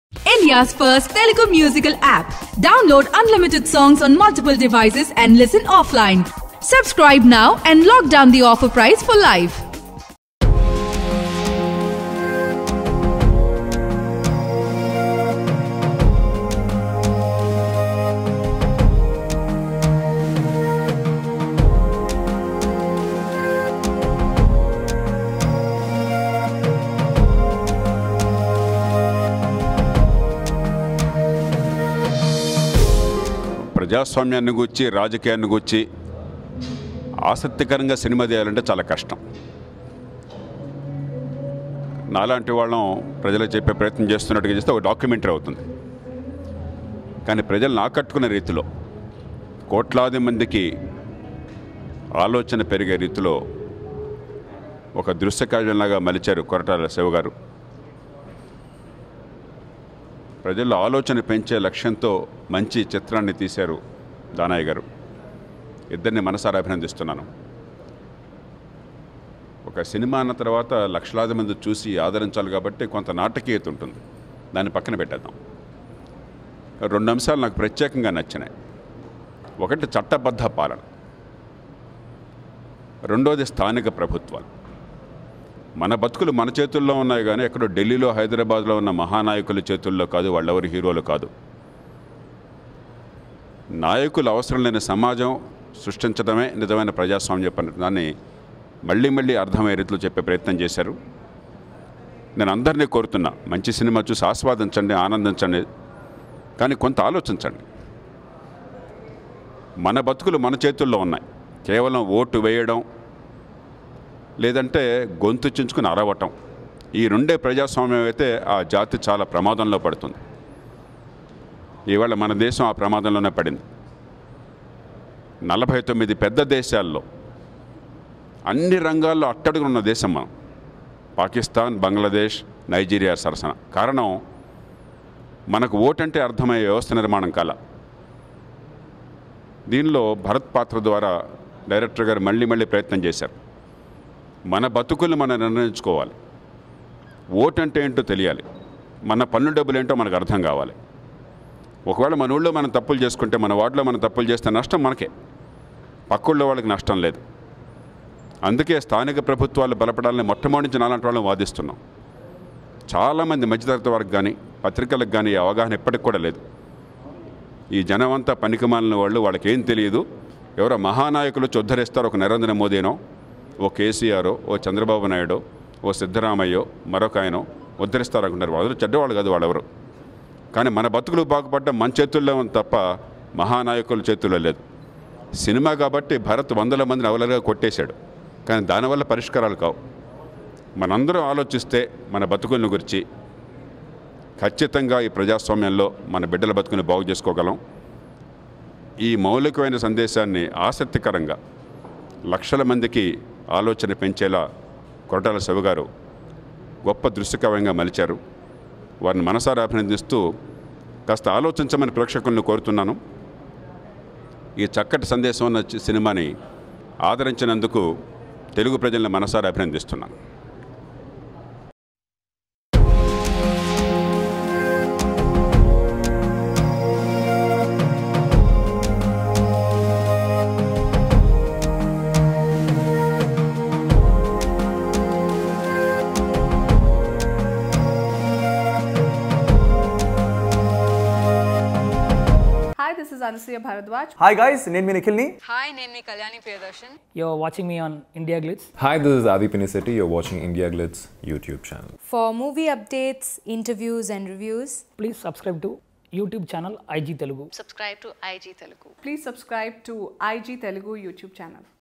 Jaya Prakash Narayan Talks about Bharath Ane Nenu and Director Koratala Siva
Jaya Prakash Narayan Talks about Bharath Ane Nenu and Director Siva

Listen unlimited Telugu songs in high quality. Download RAAGA APP. Here are download links.